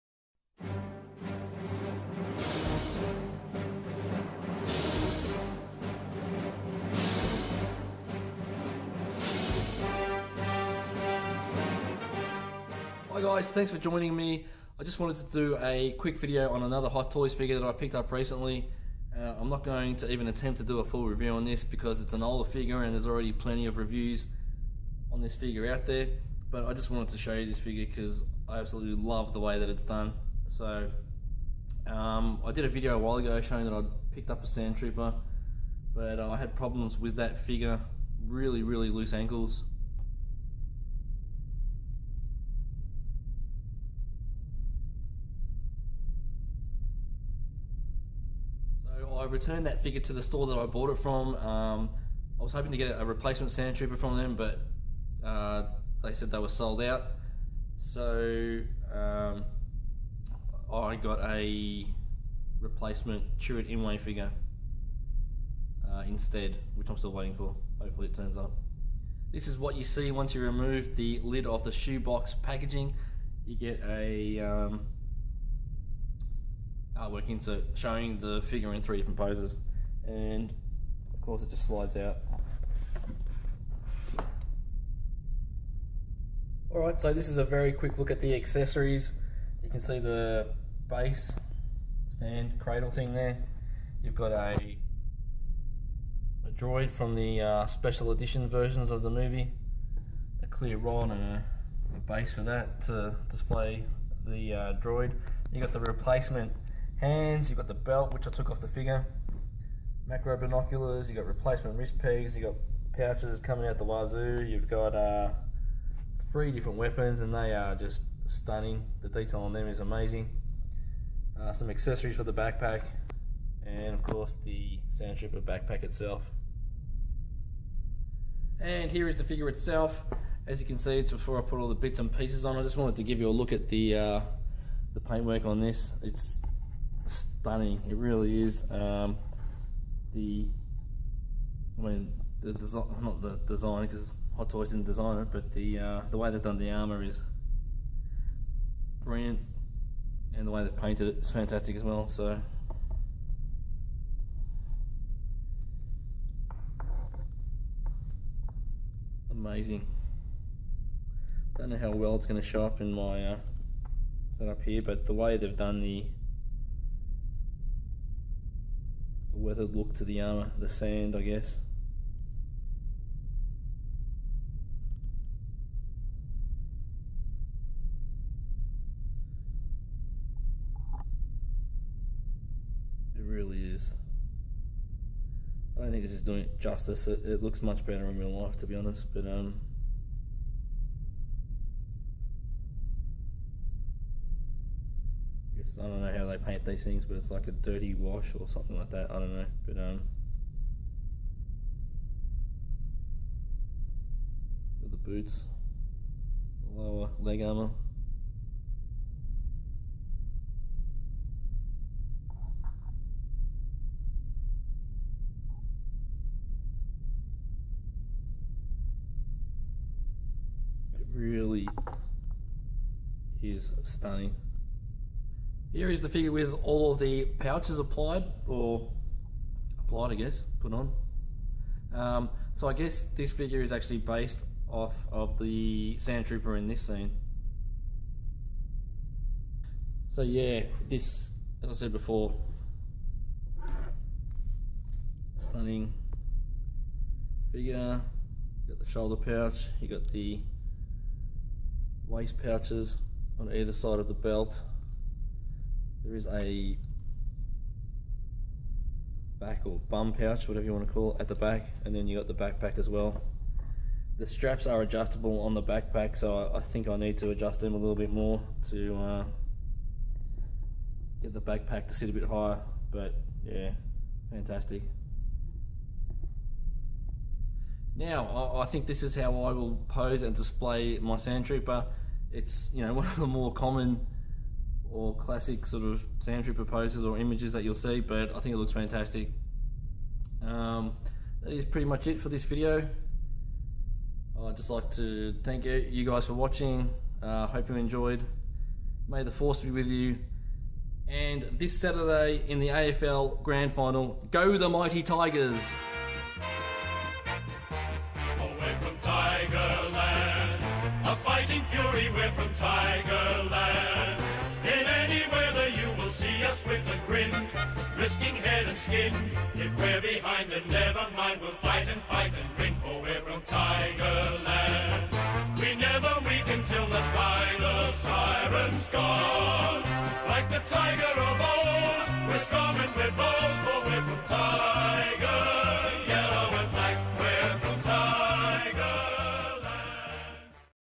Hot Toys Star Wars MMS 295 Sandtrooper review
A quick look at a fantastic Hot Toys figure.  It's been out for a while but I only just picked it up and had to share my thoughts on it.